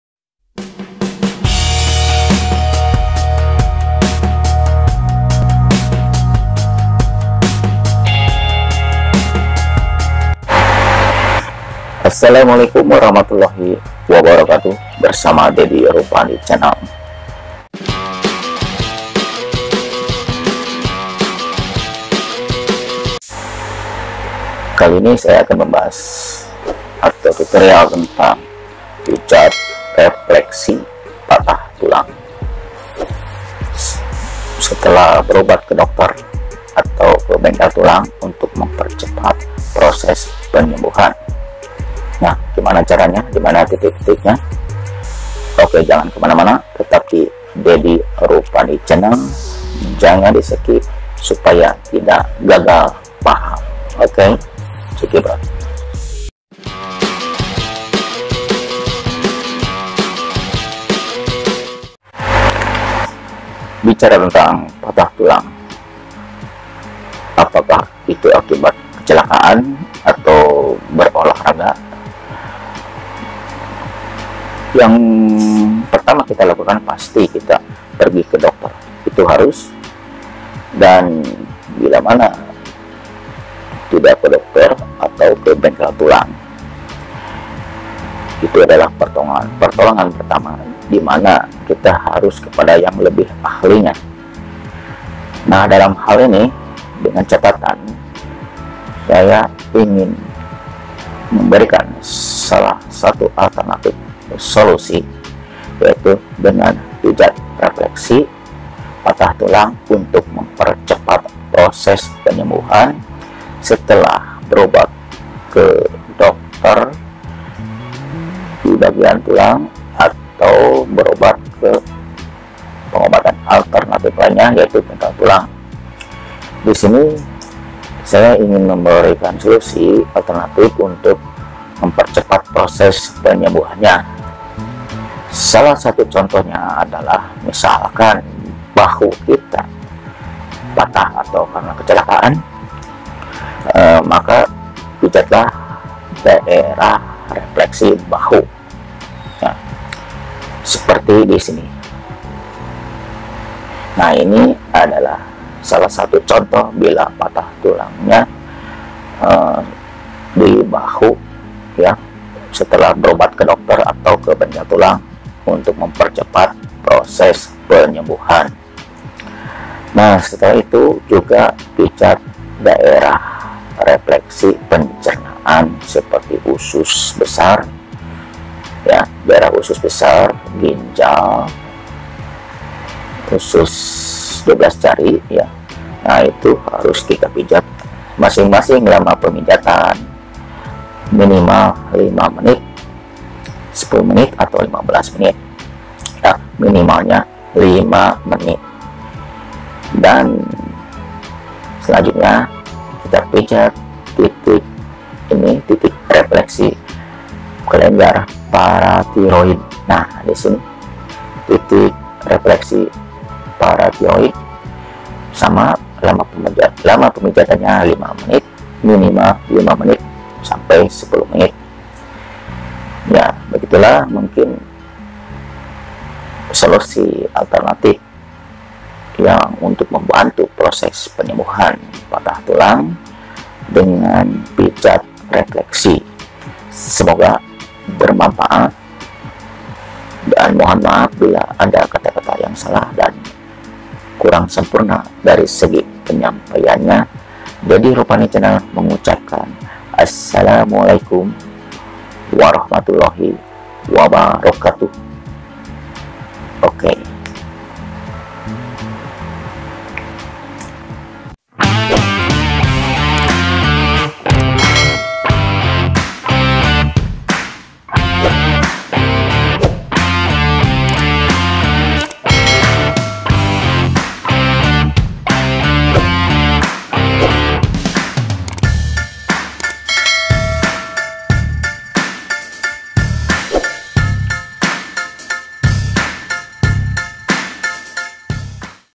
Pijat Refleksi Patah Tulang || mempercepat proses penyembuhan setelah ke dokter Tulang
Pijat Refleksi Patah Tulang || mempercepat proses penyembuhan setelah ke dokter Tulang atau ke bengkel tulang

sebagai upaya lanjutan untuk mempercepat proses penyembuhan penyambungan Tulang yang patah ,